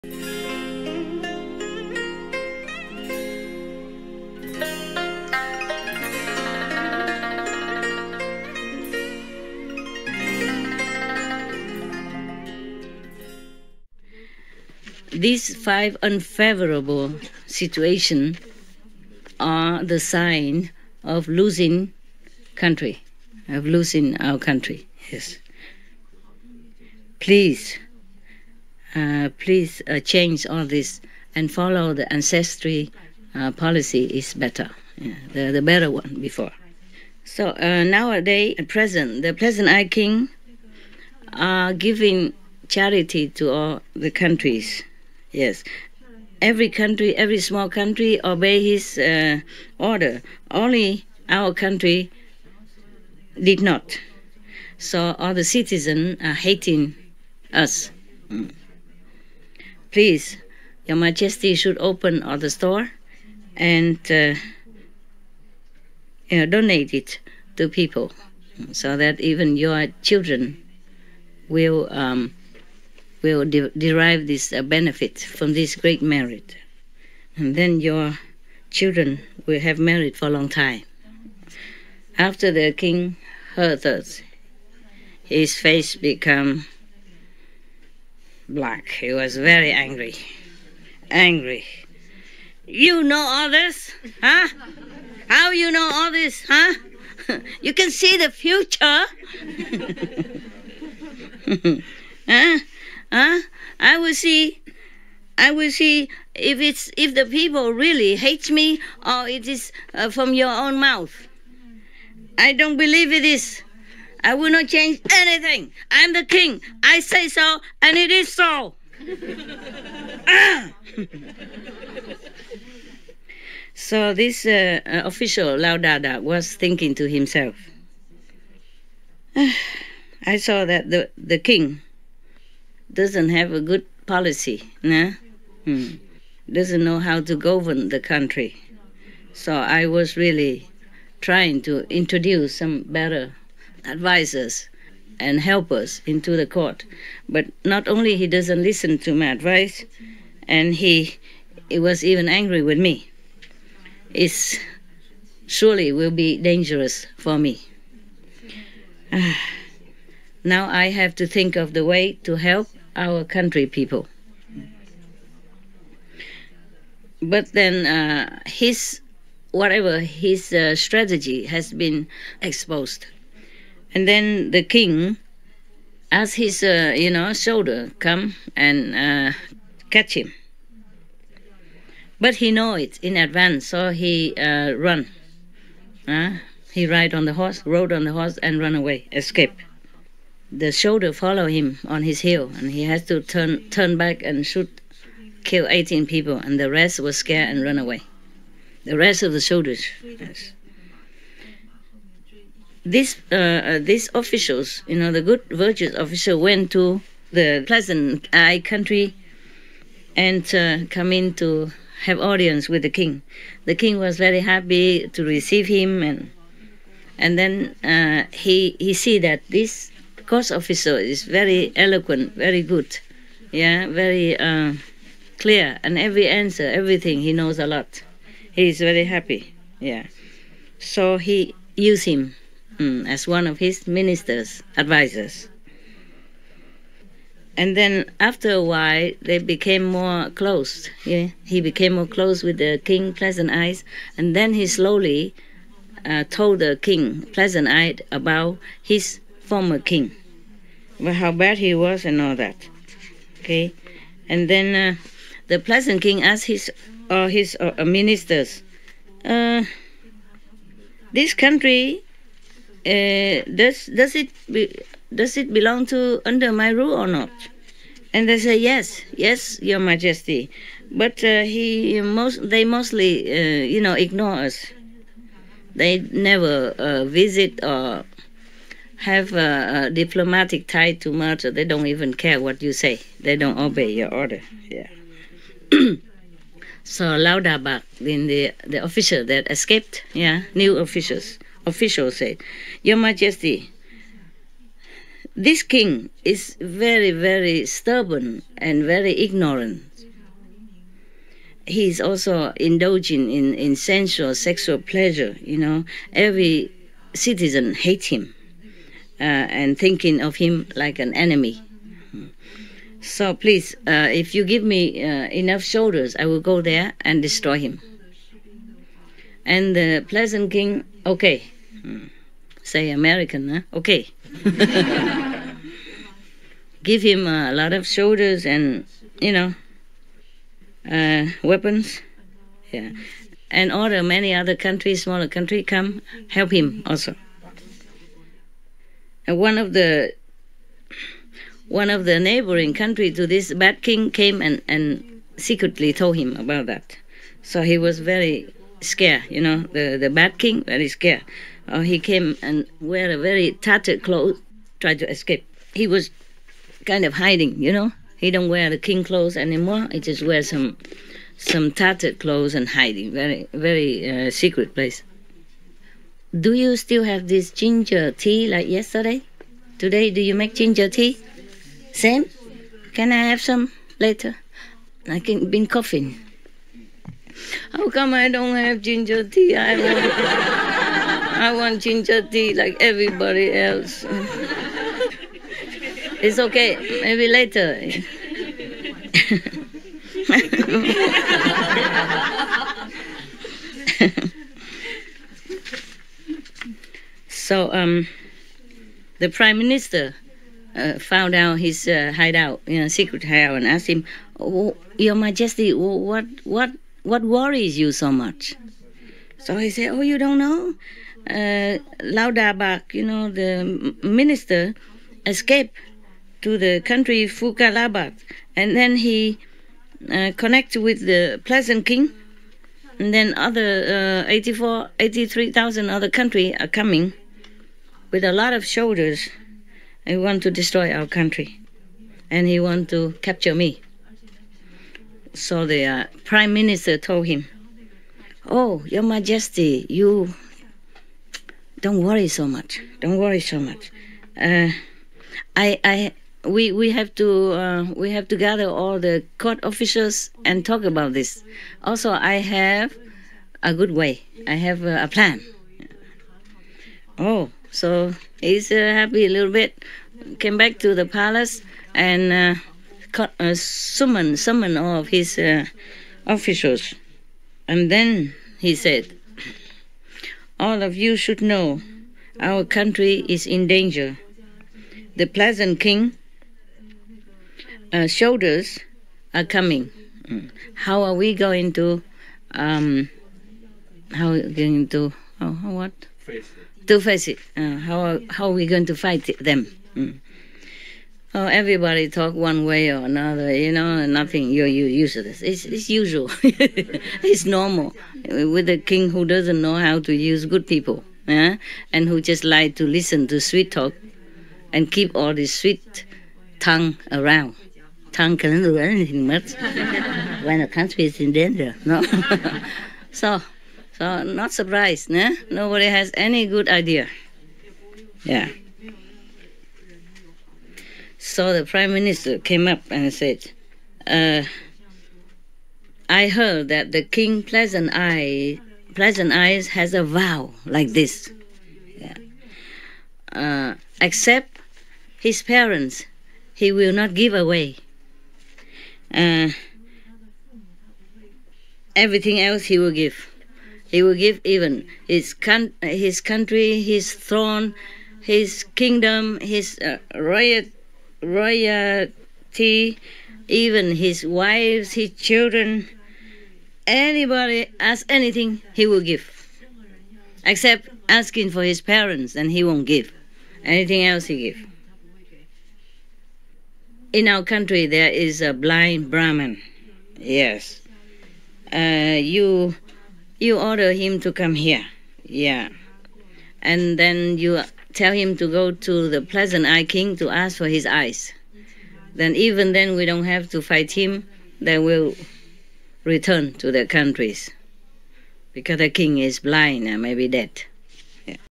(P. 2/4) The King Who Offered His Eyes to the Blind Brahmin
Supreme Master Ching's lecture,  France, Aug 27, 2018